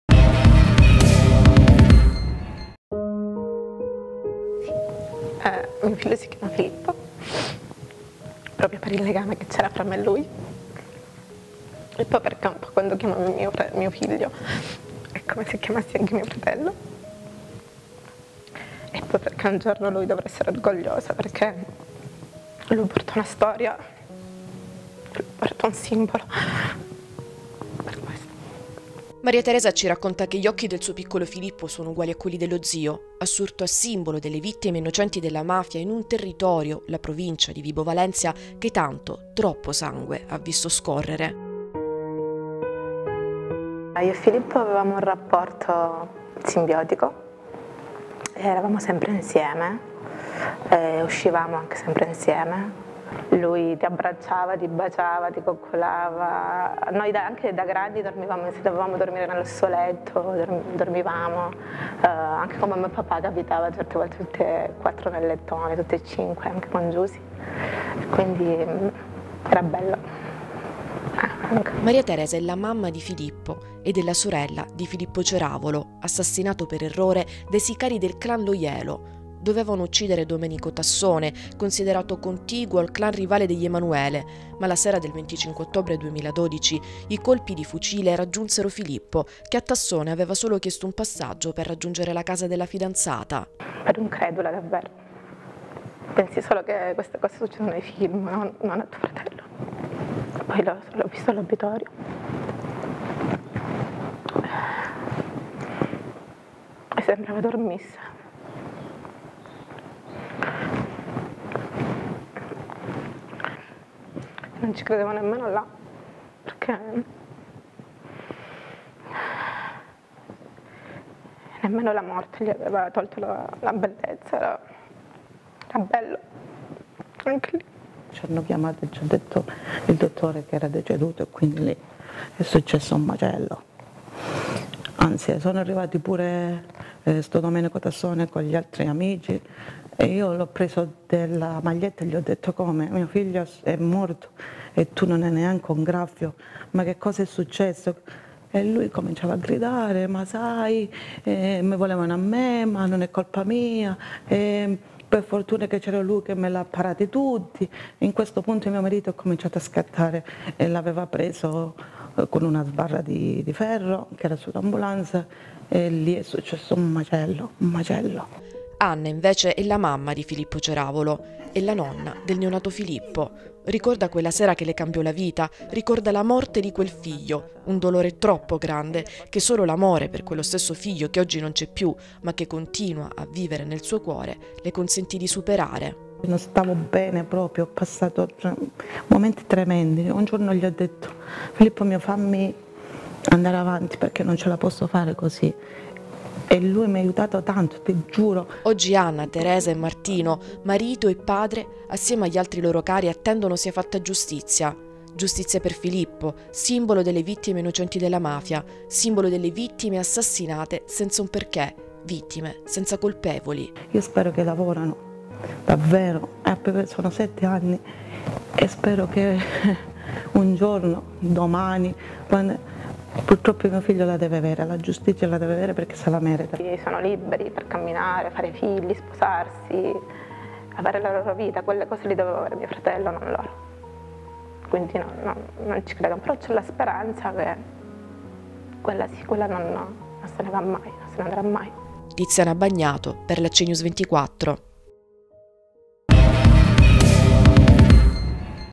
Filippo Ceravolo, la vita di una famiglia sconvolta dalla mafia
Filippo Ceravolo, vittima innocente della mafia, attende ancora giustizia. Dopo sette anni dall’agguato che gli costò la vita parla ai nostri microfoni, la sorella: ogni volta che chiamo mio figlio che si chiama come lui, dice ai nostri microfoni, una parte di mio fratello torna a vivere